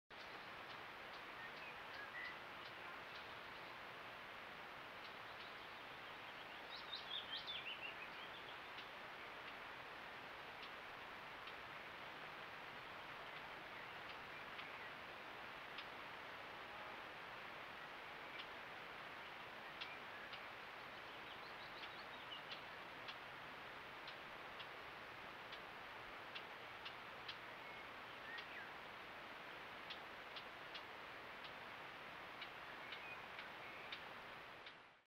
Buzzard visits a trail camera site, April 2020
Buzzard visits a trail camera site near Castletroy, Co Limerick, April 2020.